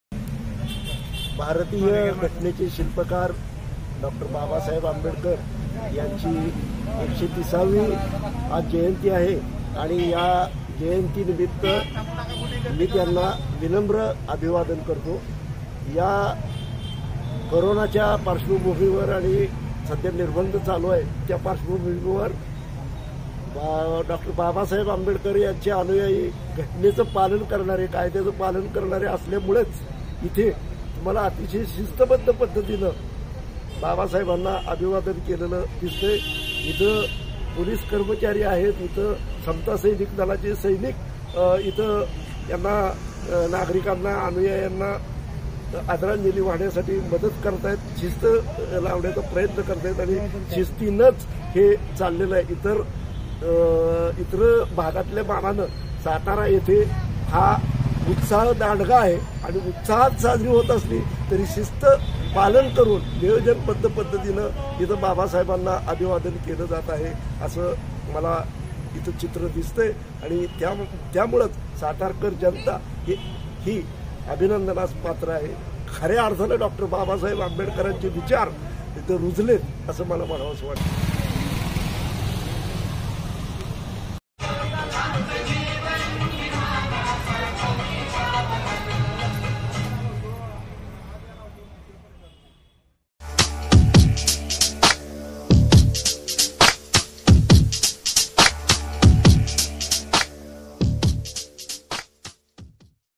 कोरोना नियमांचे पालन करत साताऱ्यात डाॅ. बाबासाहेब आंबेडकर यांची जयंती उत्साहात मात्र साधेपणाने साजरी
दि 14 एप्रिल